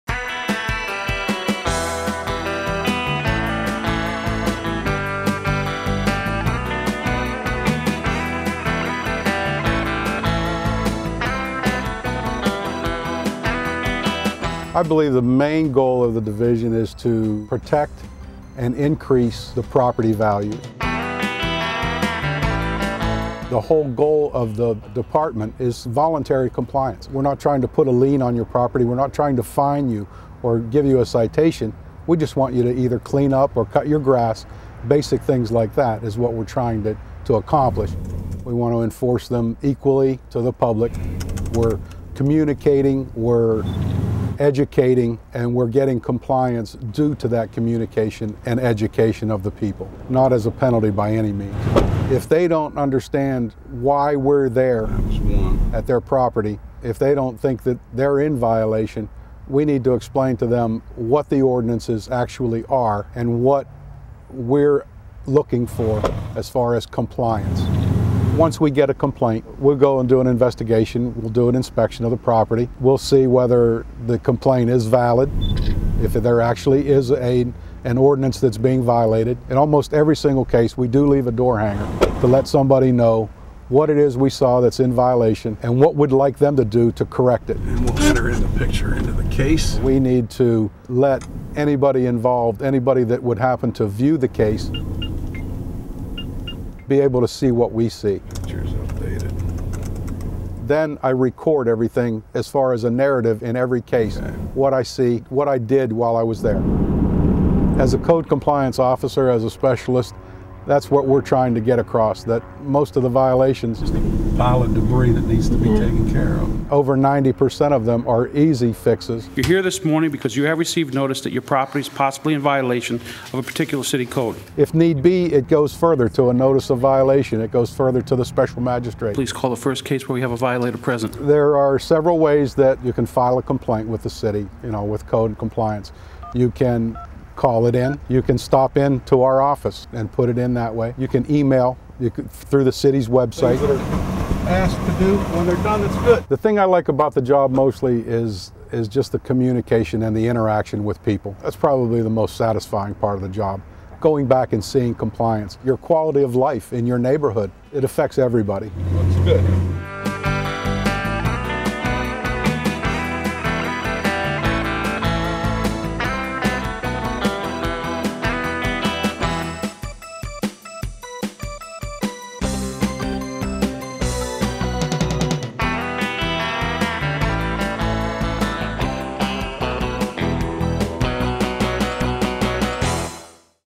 A Day in the Life of a PSL Code Compliance Specialist
The Code Compliance Division helps to protect the safety and welfare of Port St. Lucie residents, maintain and increase property values, reduce vandalism, and bolster maintenance and continued development in the city. This is a behind-the-scenes look at a typical day for Code Compliance Specialist.